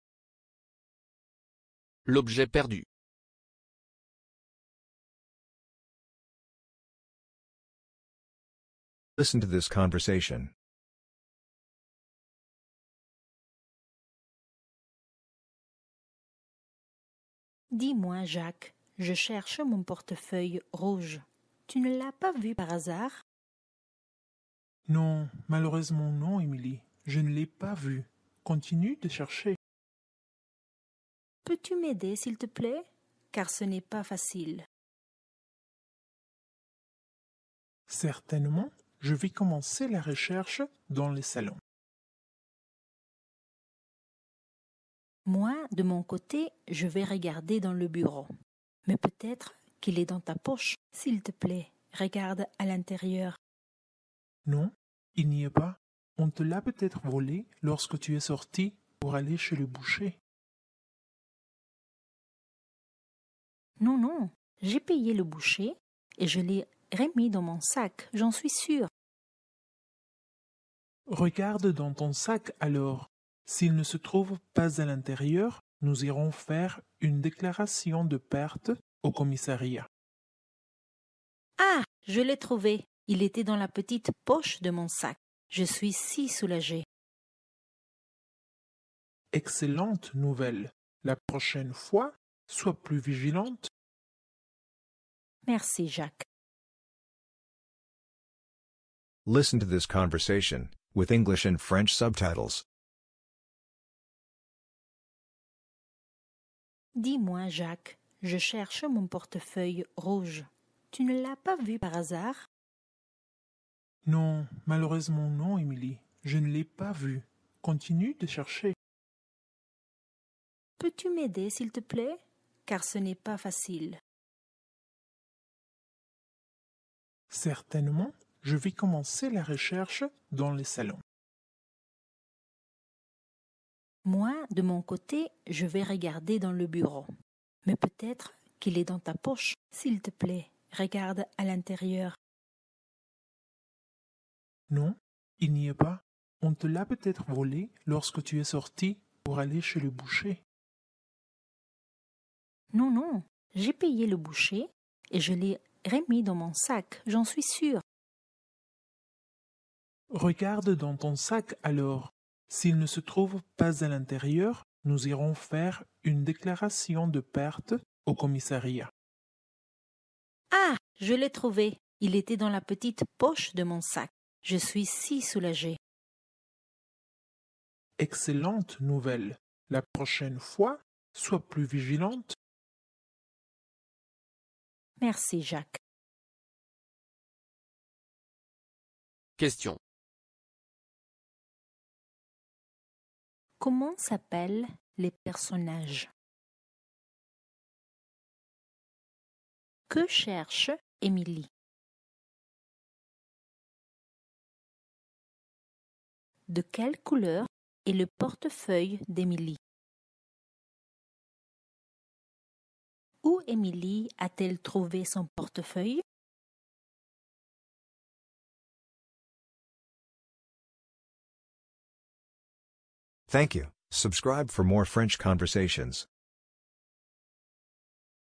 L'objet perdu Level 4 French Conversation
Practice this short conversation: “L’objet perdu” and enhance your French Vocabulary skills and make it easier to communicate.

TIMESTAMP:
00:09 Listen to the conversation (No subtitles)
01:39 Listen to the conversation (With English subtitles)
03:08 Questions

LIST OF FRENCH CONVERSATIONS IN FRENCH
SOCIAL MEDIA - HERE YOU CAN FIND US